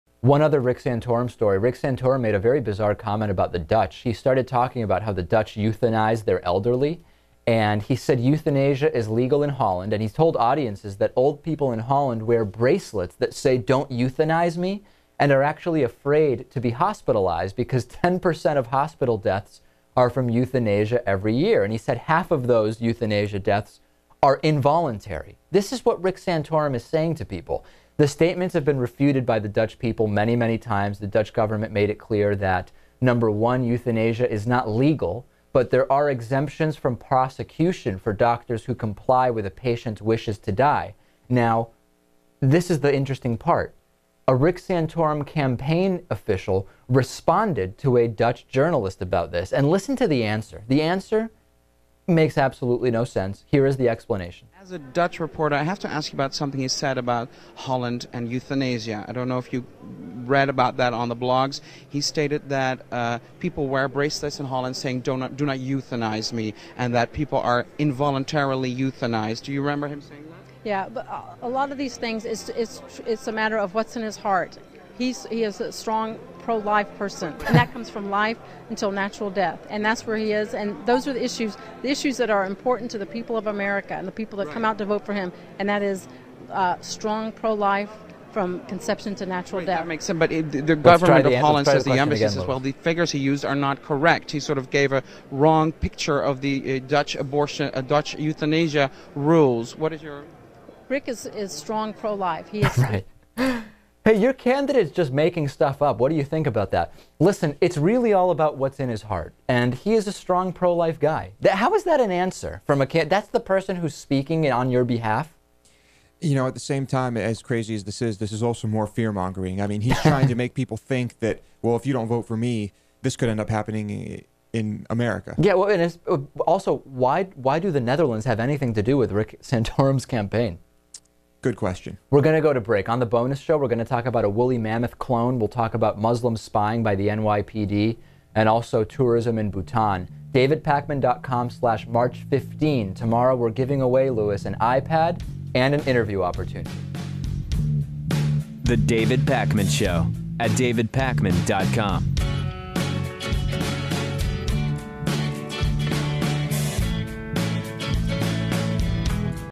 Rick Santorum Makes Stuff Up About Dutch People
--Rick Santorum's puzzling comment about Dutch euthanasia, and the even more bizarre response by a campaign spokesperson.

--On the Bonus Show: Wooly mammoth clone, NYPD & Muslim spying, Bhutan and tourism, and more.

24/7 Voicemail Line: (219)-2DAVIDP